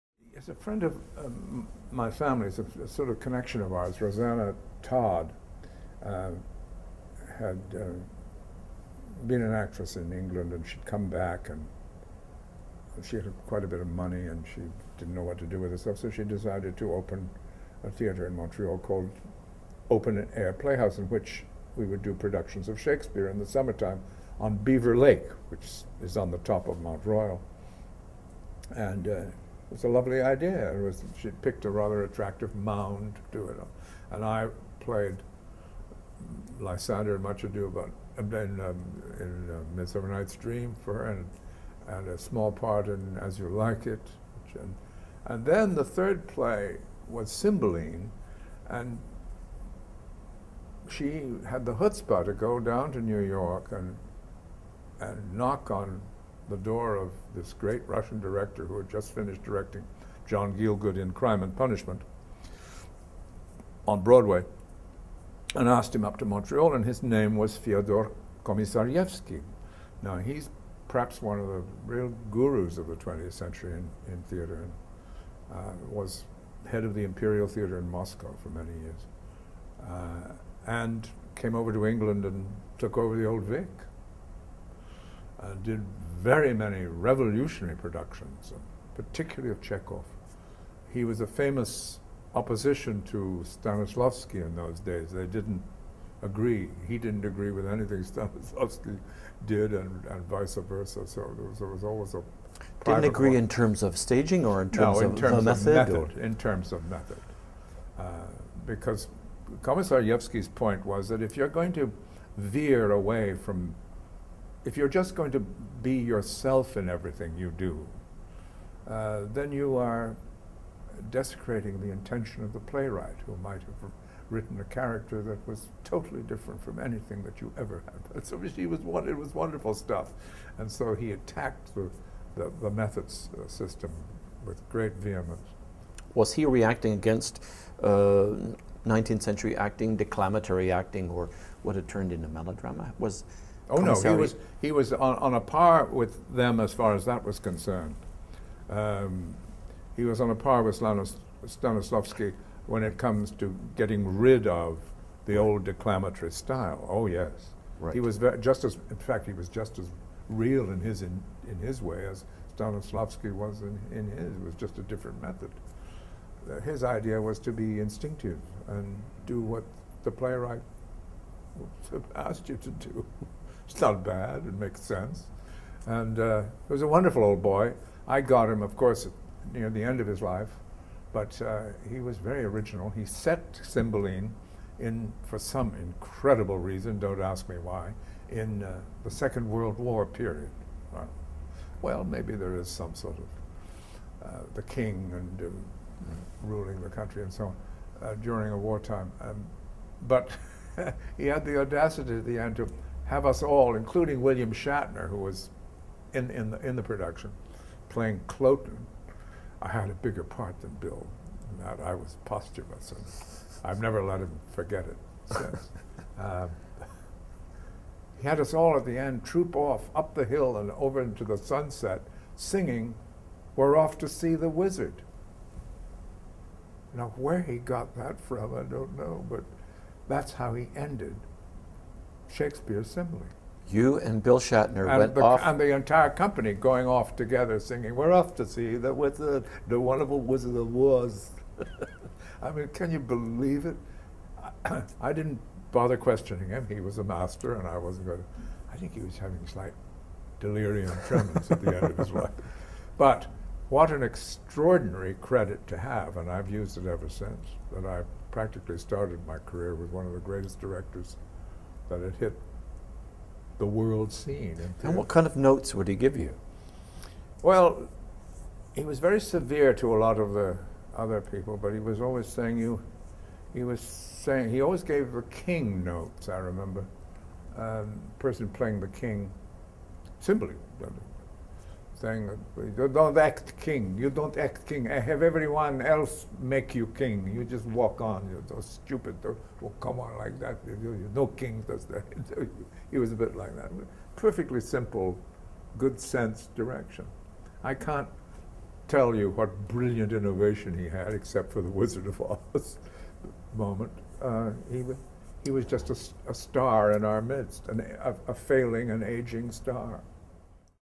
Christopher Plummer on Fyodor Komisarjevsky (Part 8 of 44)
Christopher Plummer on working with an innovative Russian director.

Interviewed by R.H. Thomson on October 6, 2007 in Toronto. Filming location courtesy of Four Seasons Hotel. Generously supported by The William and Nona Heaslip Foundation.